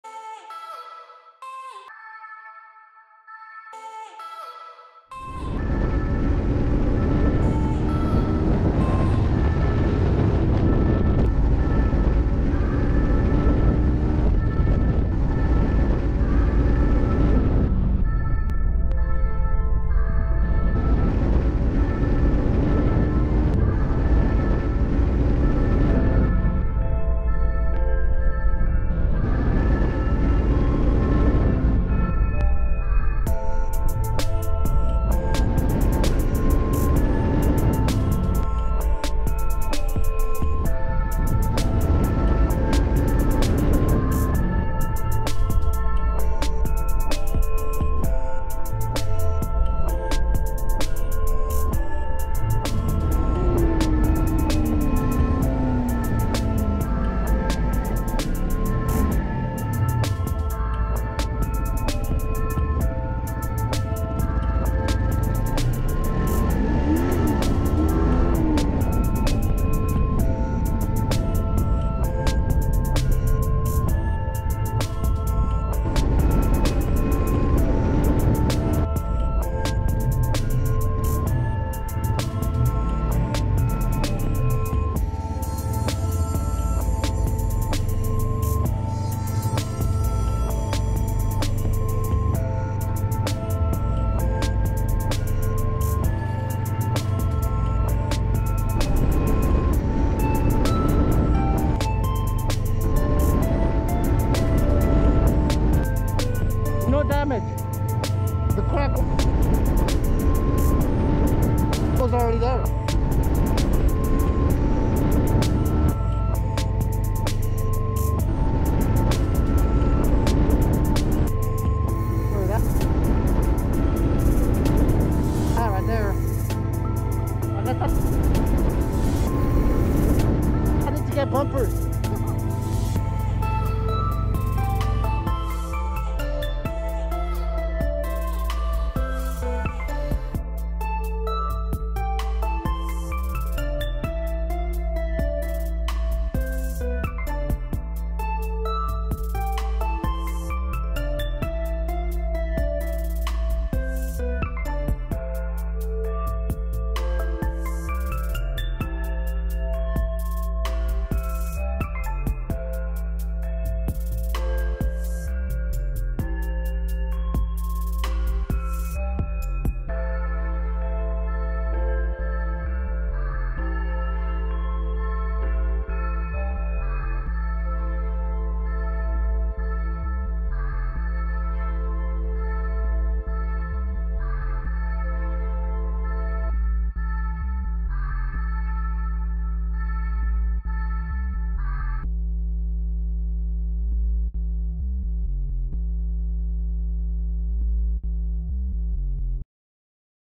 iPhone Durability Test
When out for a ride in in the Texas sun an accident was in the stages of being captured.